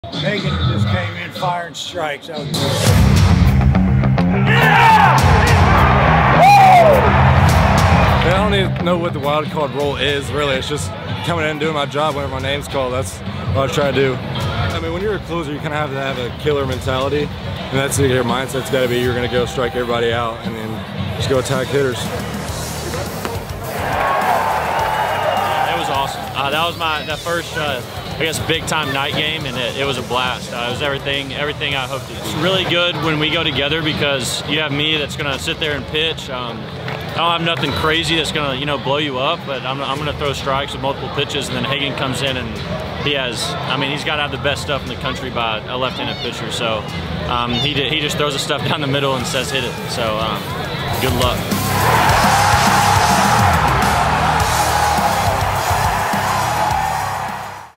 Game 1 to the Hogs! Arkansas beats Tennessee, 5-2
Razorback Access: Hollan & Smith Shut Down Tennessee

Exclusive insider access with the Razorback baseball team after their big Game 1 win against Tennessee. Hear Dave Van Horn's message to the team after plus one-on-one interviews with stars Hagen Smith, Hunter Hollan, and Tavian Josenberger.